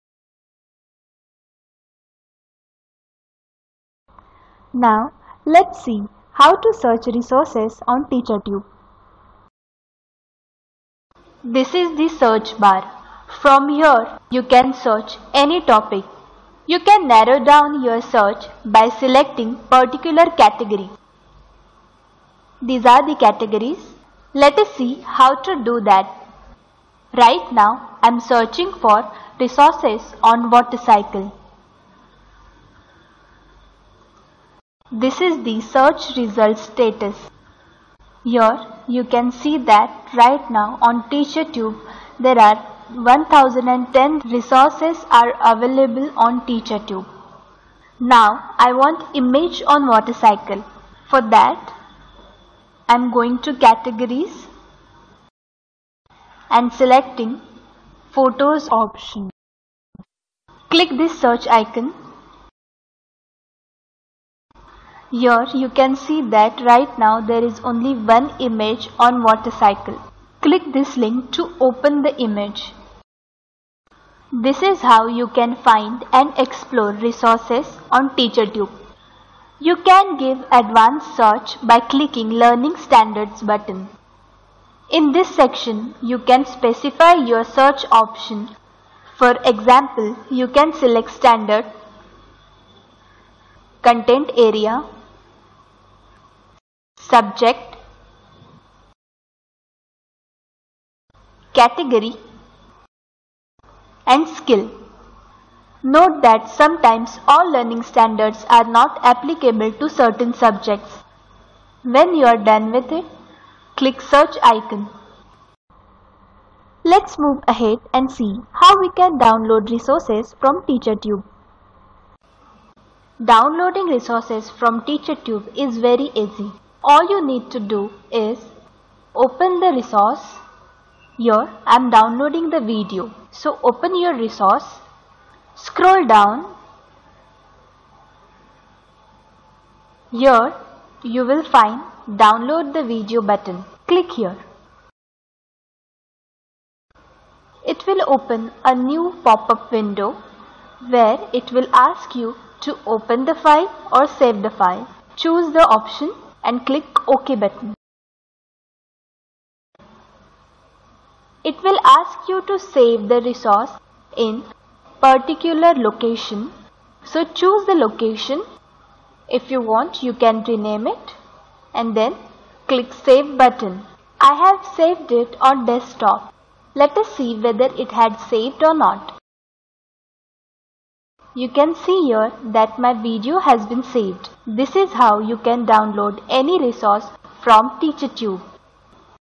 4 TeacherTube Search and Download
This video explains search and download function of TeacherTube.